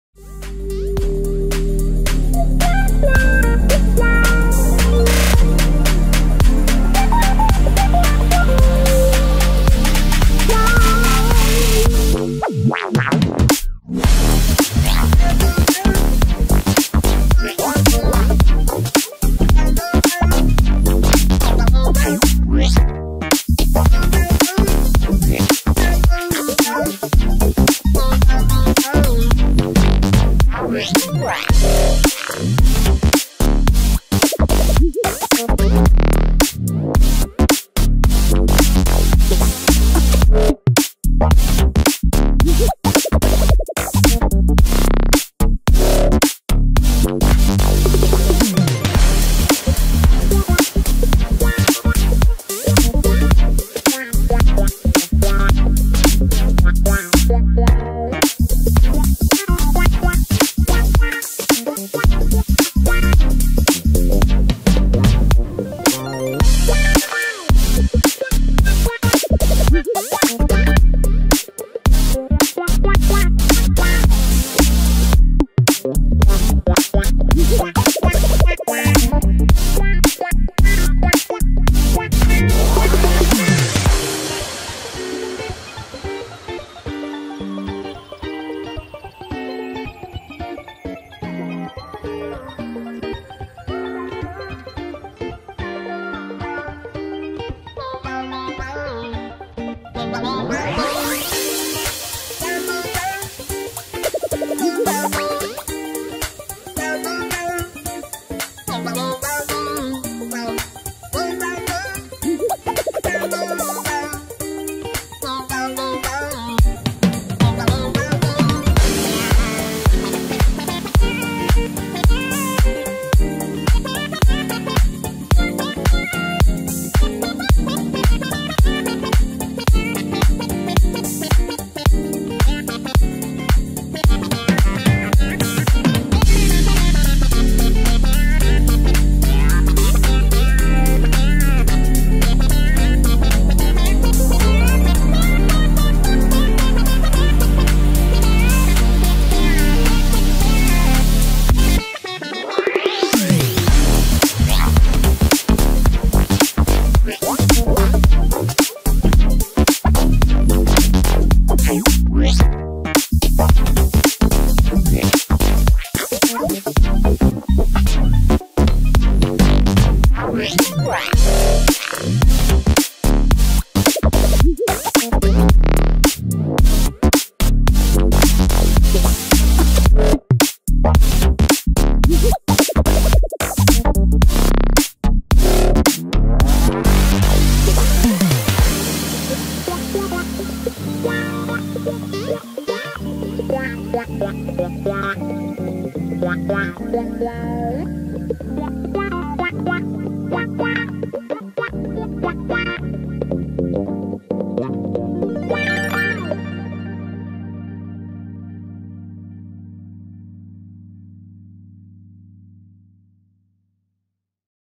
Matheus Cunha Atacante 2019 B
Mais algumas jogadas e performance do Matheus Rocha Pedreira de Araujo Cunha em 2019.